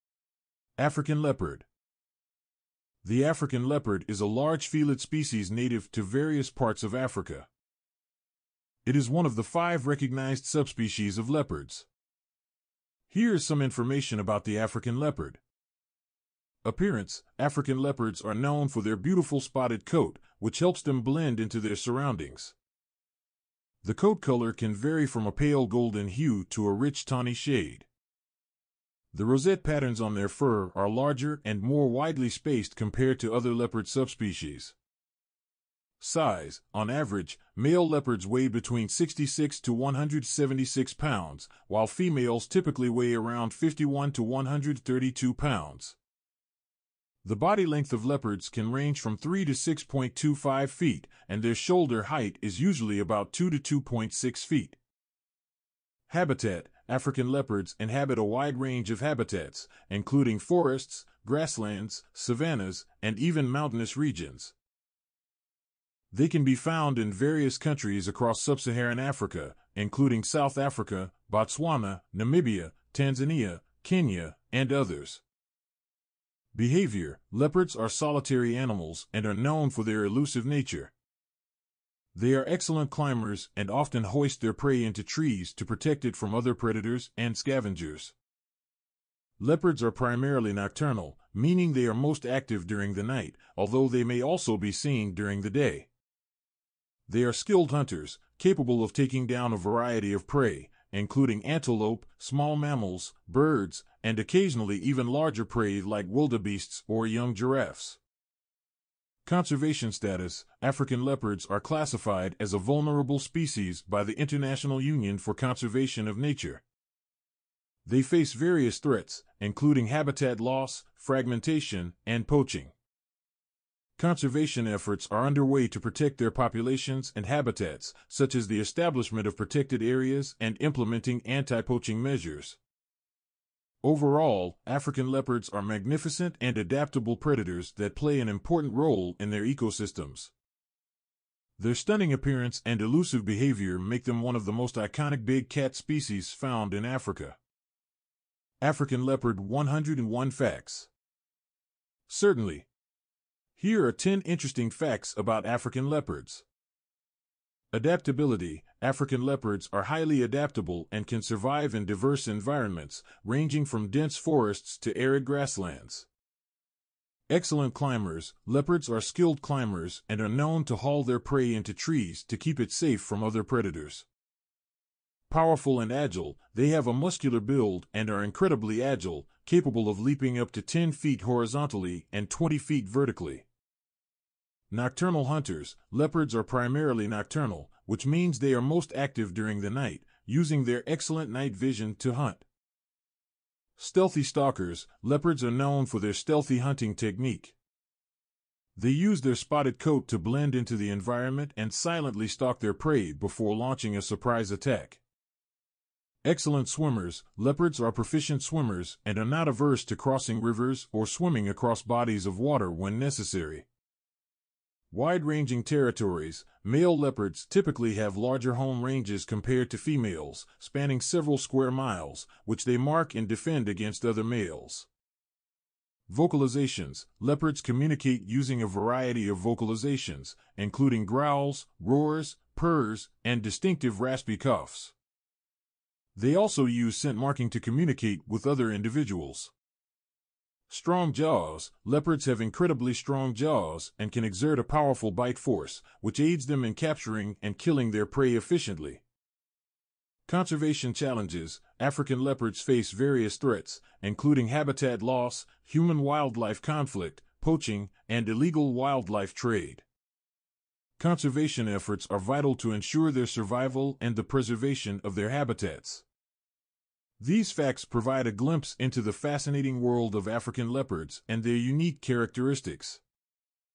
African Leopard 101 Facts - Personality - Speed - Health cuteanimals Video
African Leopard 101 Facts - Personality - Speed - Health

The African leopard (Panthera pardus pardus) is a large felid species native to various parts of Africa. It is one of the five recognized subspecies of leopards. Here is some information about the African leopard:

Appearance: African leopards are known for their beautiful spotted coat, which helps them blend into their surroundings. The coat color can vary from a pale golden hue to a rich tawny shade. The rosette patterns on their fur are larger and more widely spaced compared to other leopard subspecies.